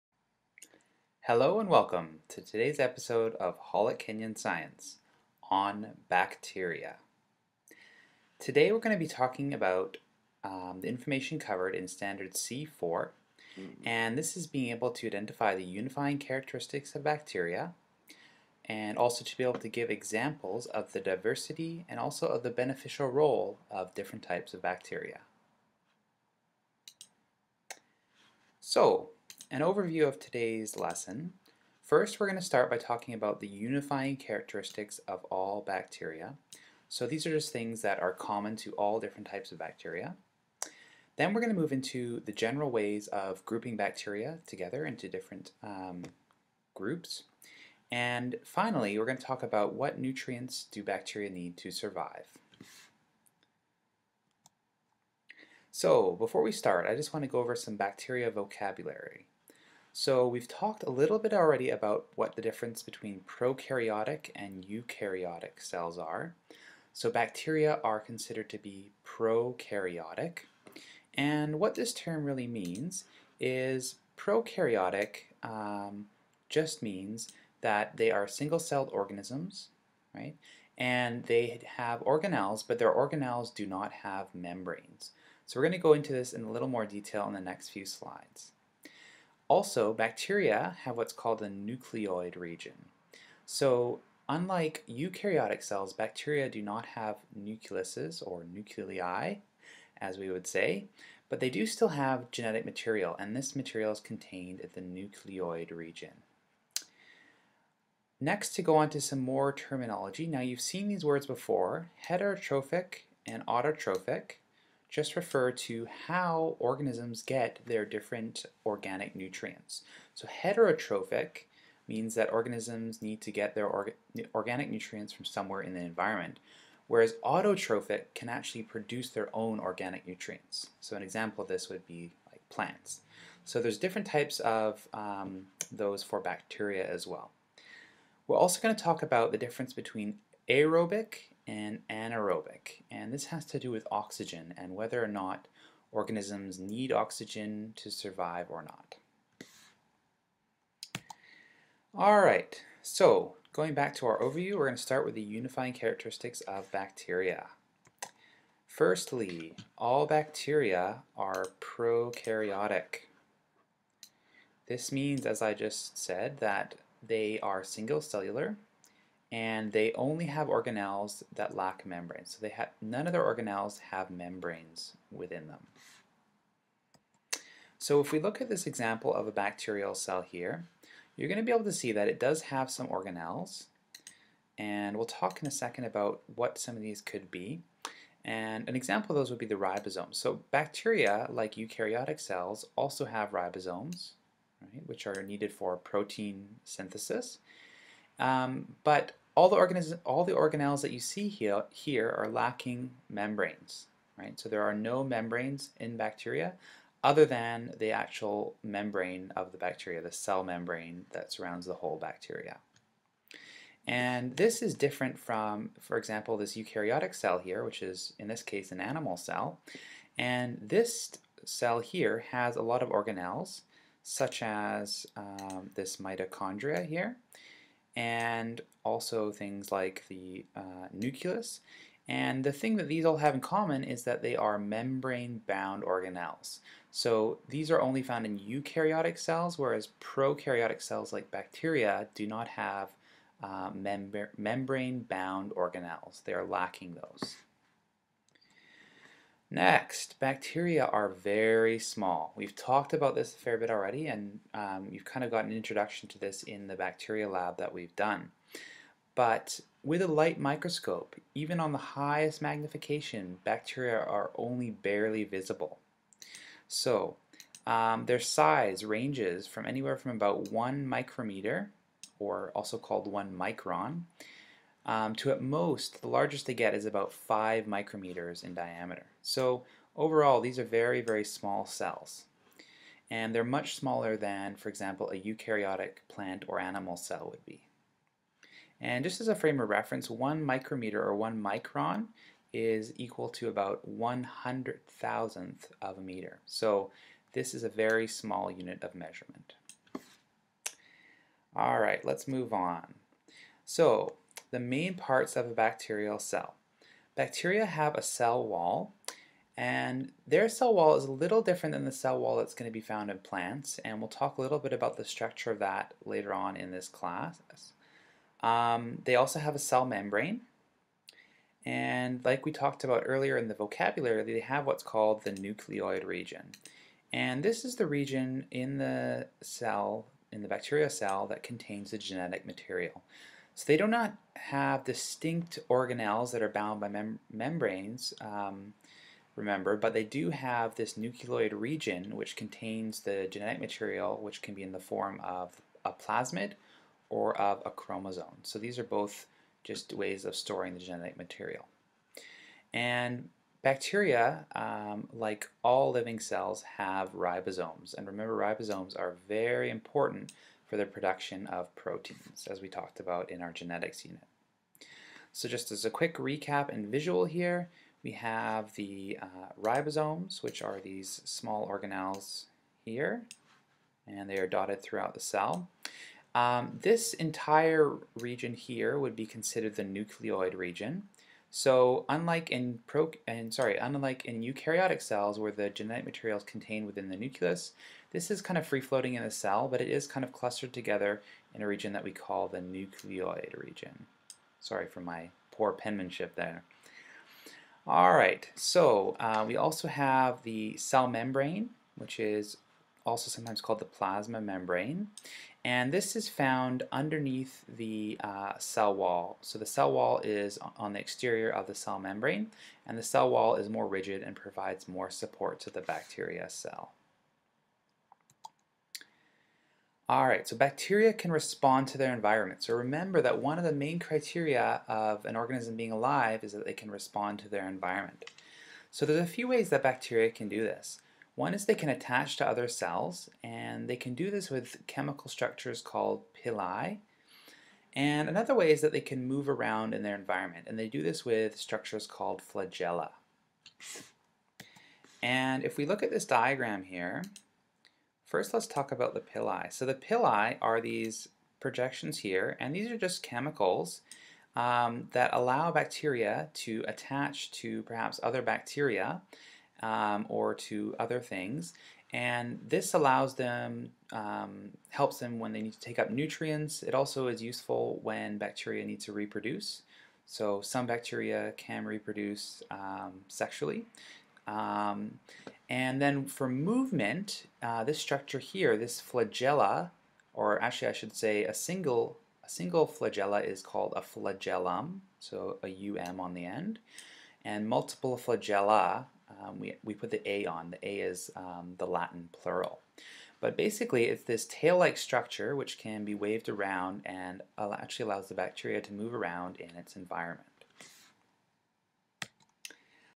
Bacteria Part 1.mp4
Table of Contents:

00:00 - Bacteria
00:26 - Overview:
00:54 - Bacteria Vocabulary:
02:30 - All Bacteria are Prokaryotic
02:32 - Bacteria Vocabulary:
02:33 - Overview:
02:36 - Bacteria Vocabulary:
02:37 - All Bacteria are Prokaryotic
02:56 -
04:08 - Bacteria are very small
05:02 - Main parts of a bacteria cell:
06:04 -
06:58 - Responding to their Environment
07:24 -
08:22 - Overview: